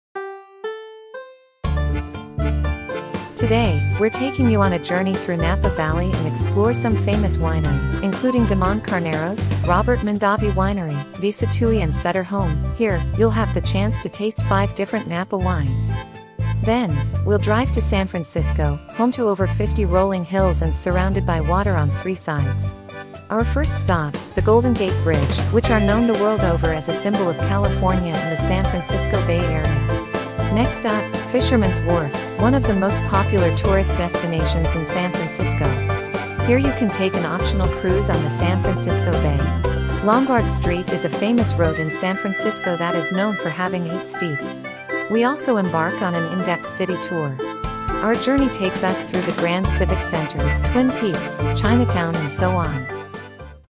Sonoma-Napa Valley-San Francisco
Today, we will travel to Napa Valley, a world-famous wine producing area, and enjoy an exclusive itinerary to drink Napa wine. Then head to San Francisco, which is surrounded by the sea on three sides. We will visit Golden Gate Bridge, Fisherman's Wharf, Lombard Street. Also take a in-depth San Francisco tour including Civic Center, Twin Peaks, Chinatown and so on.